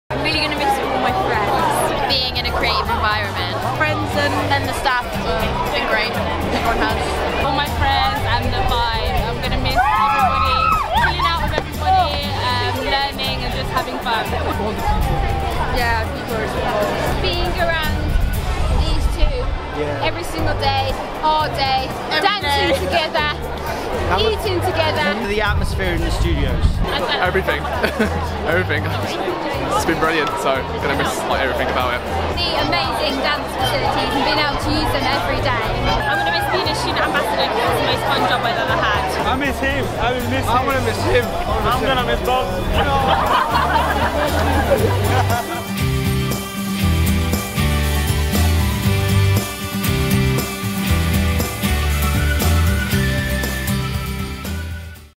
What will you miss most about DMU?
We asked graduates what they'll miss most about DMU...